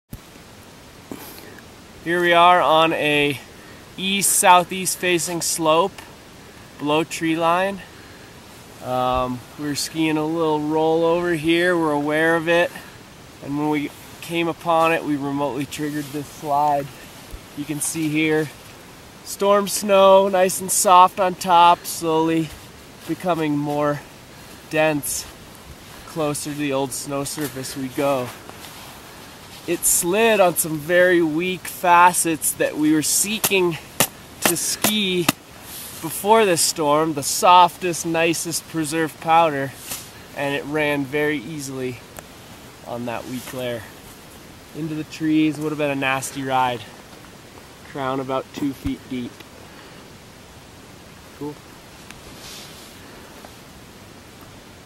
1/31/2014 Remotely Triggered Avalanche near Anthracite Mesa
Short video by CBAC forecaster Ian Havlick investigating a SS-ASr-R4-D2-O  ESE facing slope on steep rollover near 10,000ft in the Crested Butte, CO backcountry.